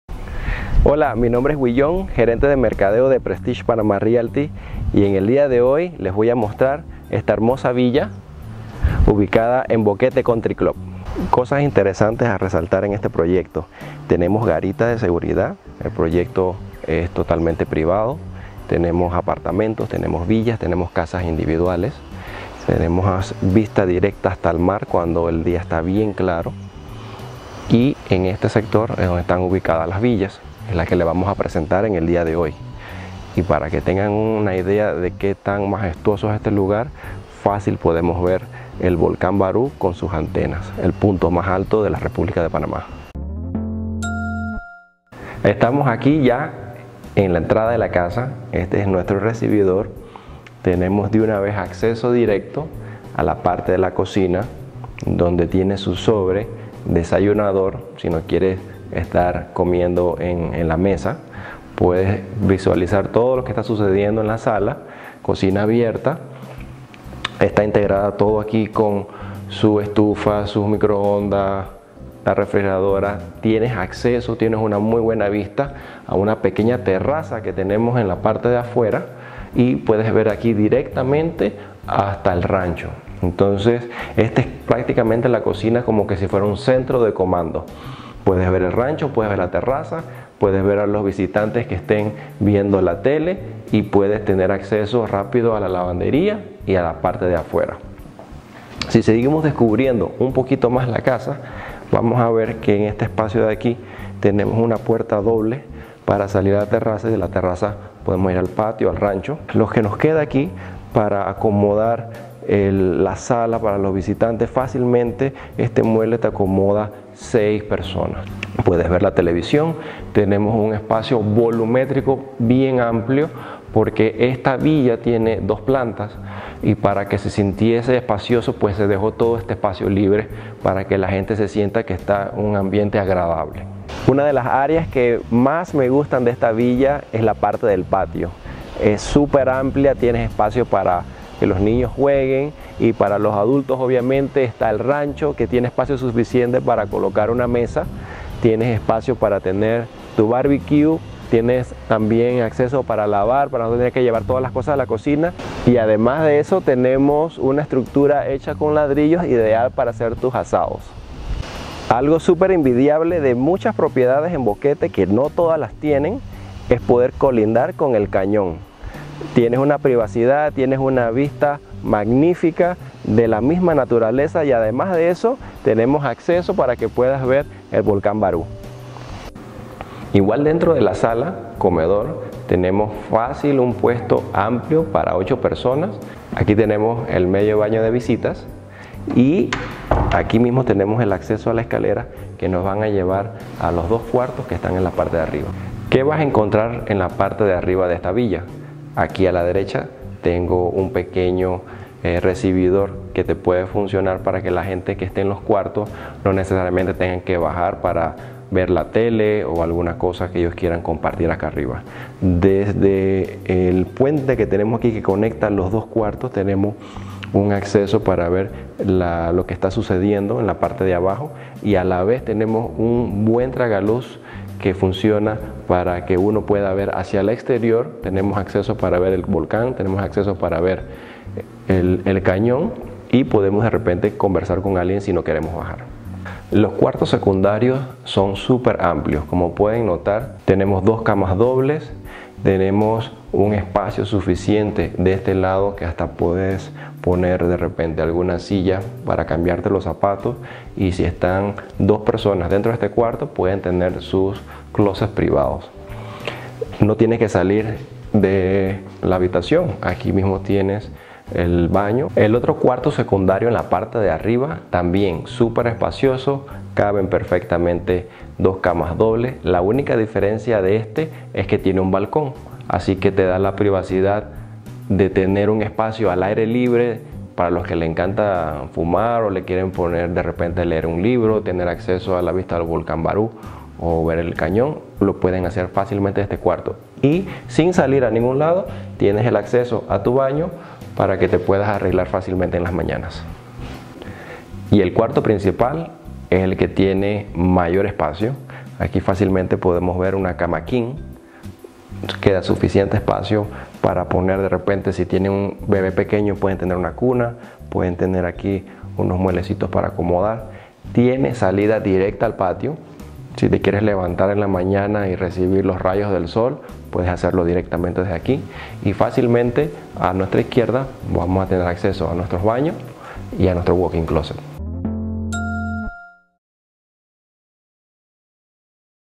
Alquiler/Venta de casa AMOBLADA en Urb. Boquete Country Club. Prestige Panama Realty. 6981.5000
Alquiler/Venta de casa AMOBLADA en Urbanización Boquete Country Club, Chiriquí
Rent/Sale of FULLY FURNISHED house in Boquete Country Club Urbanization, Chiriquí

ID: 1502  |  Tipo: Apartamentos, Casas, Villas

Precio (USD): Venta $450,000 - Alquiler, $2,500

Dirección: Chiriquí. Urbanización Boquete Country Club

Cant. de Habitaciones: 3  |  Cant. de Baños: 3.5  |  Mascotas permitidas: No  |  Amoblado(a): Sí  |
Descripción:
Alquiler/Venta de propiedad completamente amoblada en Urbanización Boquete Country Club.

Esta casa es amplia, de dos niveles con un área total de 255 m2. Cuenta con 3 recámaras, cada una con su baño y un medio baño para visitas en la planta baja de la casa.

Al adquirir esta propiedad podrán disfrutar de los beneficios de la Urbanización como el área social con sauna, piscina, gym, sala de eventos y jacuzzi.

La casa cuenta con una cocina abierta, equipada con todo lo que necesitas, amplia sala con un espacio abierto gracias a su techo doble altura, y la recámara principal en planta baja.

Lavandería cerrada, terraza, área de BBQ para compartir momentos únicos en el hogar. La propiedad cuenta con un jardín organizado y colindante con el cañon brindando a esta propiedad una vista hermosa y clima muy agradable.

La urbanización brinda una garita de seguridad y estacionamientos de visitas y privados.
Comodidades:
Estacionamiento(s) bajo techo
Jardín
Área para desayunador
Calle sin salida
Área Social
Lavandería cerrada
Abanico de techo
Cocina con desayunador
Agua potable
Área de BBQ
Área de juego para niños
Línea blanca
Cocina abierta
Derecho de uso al club house

ID: 1502  |  Type: Apartment, House, Villa

Price (USD): Sale $450,000 - Rent, $2,500

Total Area: 1,935 ft2  |  Construction Area: 820 ft2  |  Lot Area: 1,935 ft2  |

Address: Chiriquí. Boquete Country Club Urbanization

Bedrooms: 3  |  Bathrooms: 3.5  |  Pets allowed: No  |  Furnished: Yes  |
Description
Rent / Sale of fully furnished property in Boquete Country Club.

This house is spacious, on two levels with a total area of 255 m2. It has 3 bedrooms, each with its own bathroom and a half bathroom for visitors on the ground floor of the house.

By acquiring this property you can enjoy the benefits of the Urbanization as the social area with sauna, swimming pool, gym, events room and jacuzzi.

The house has an open kitchen, equipped with everything you need, large living room with an open space thanks to its double height ceiling, and the main bedroom on the ground floor.

Closed laundry, terrace, BBQ area to share unique moments at home. The property has an organized garden adjoining the canyon giving this property a beautiful view and very pleasant climate.

The urbanization provides a security checkpoint and visitor and private parking.
Amenities:
Covered parking lot
Garden
Breakfast area
Dead end street
Social Area
Enclosed laundry
Ceiling Fans
Kitchen: with breakfast area
Drinking water
BBQ area
Playground
Appliances
Open kitchen
Access to club house